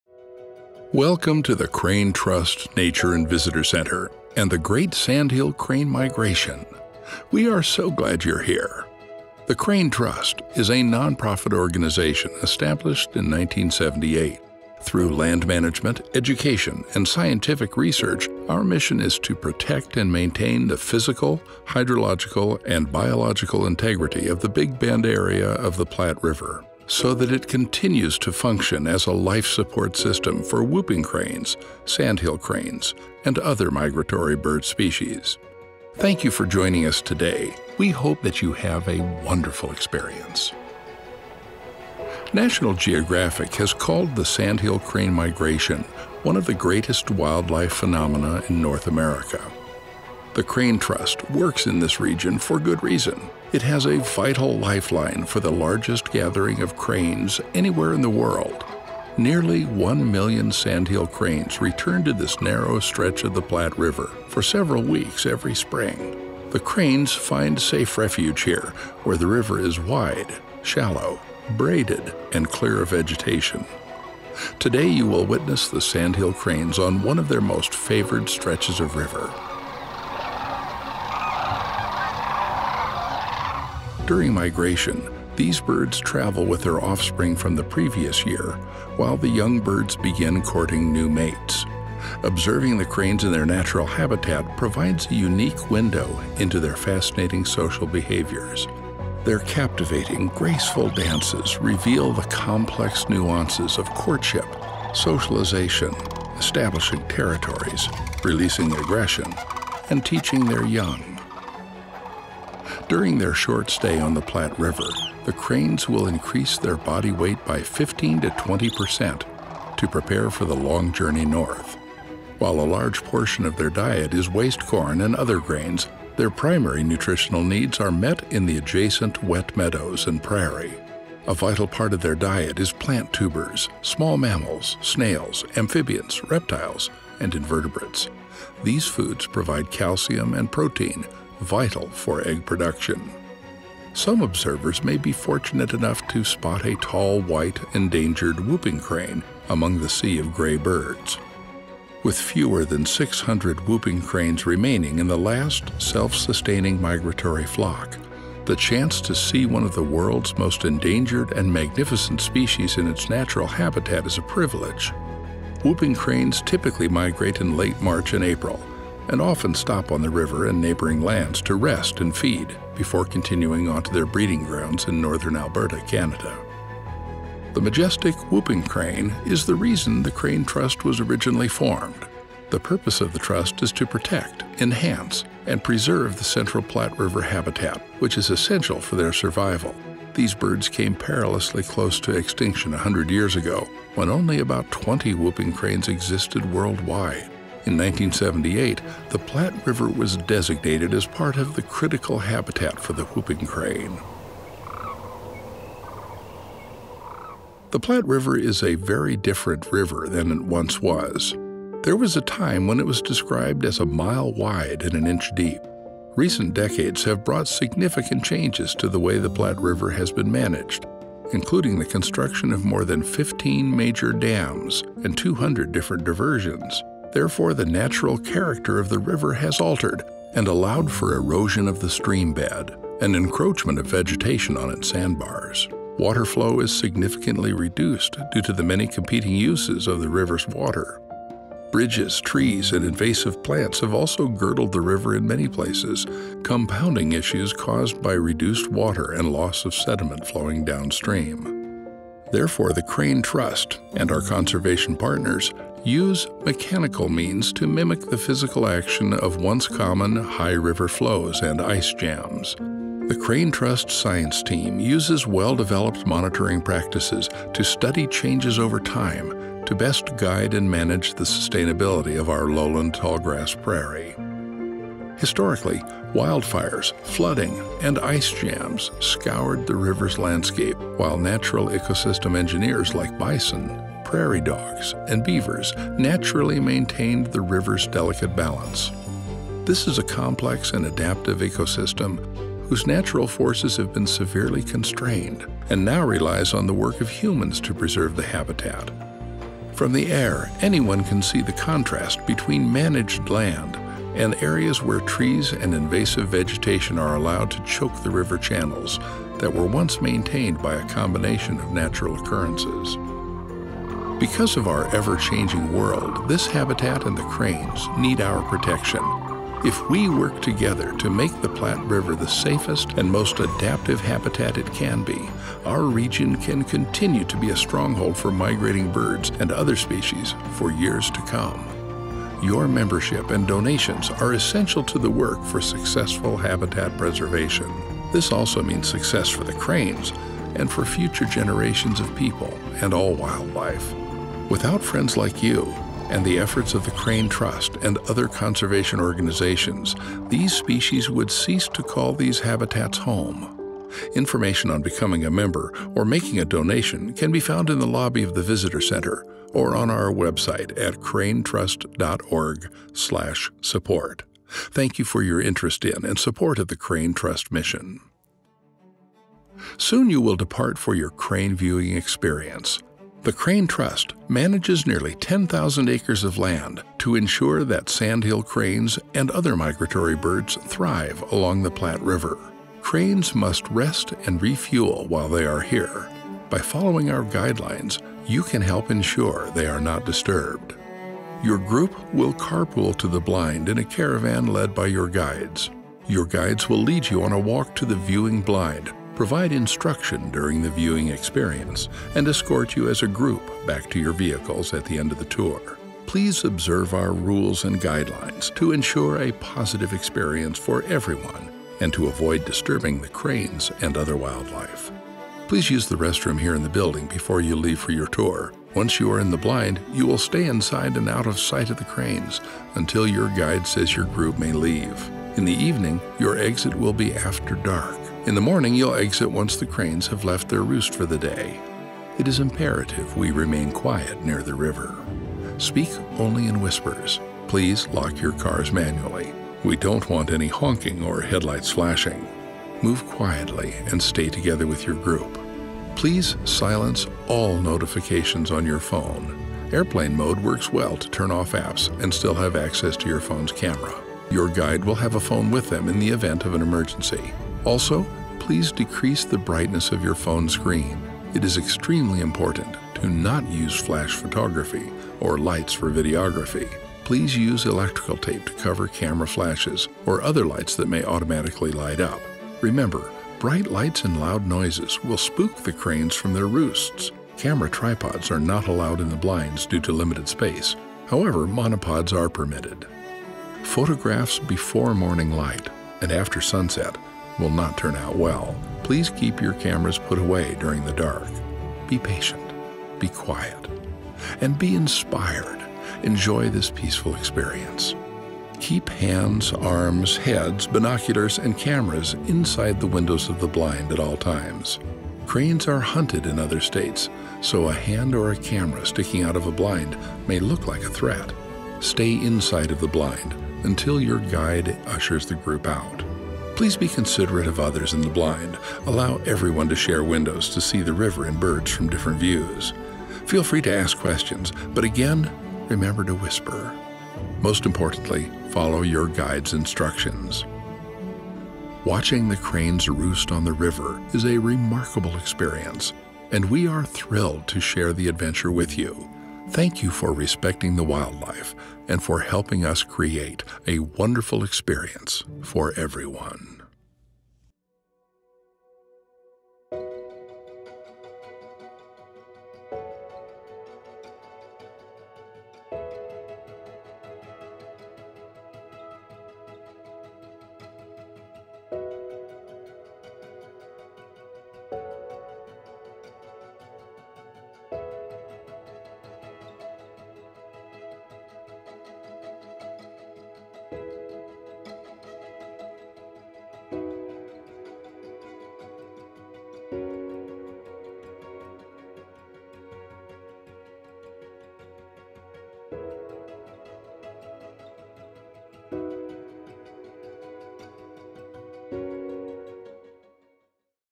Riverfront Blind Tour: Migration, Conservation, and Viewing Etiquette
Every year, Sandhill Cranes and Whooping Cranes stop in the Central Platte River Valley during their migration. This video explores why they come here, where they’re going, and how the Crane Trust works to protect their habitat. It also covers the history of the Platte River, the changes to its landscape, and why active management is necessary to maintain its ecological integrity.

For those joining a Riverfront Blind Tour, this video provides important guidelines to ensure a respectful and enjoyable viewing experience. Guests should remain mindful of noise, light, and movement to avoid disturbing the cranes.